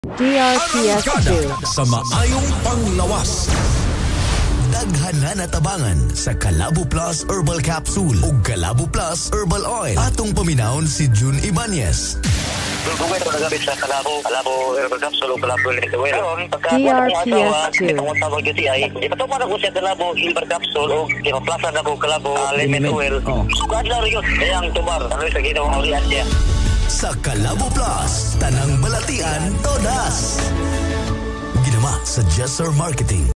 Kalabo Plus Radio Ad 2023-2024 33s (Testimonial Version 4)
Ito po ang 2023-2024 Radio Ad ng Kalabo Plus. (Testimonial Version 4)

CORRECTION: The advertiser is "Jes-Ser Marketing", not "TMJM Marketing".

DRPS Two 8242: Kalabo Plus Radio Ad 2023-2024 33s (Testimonial Version 4)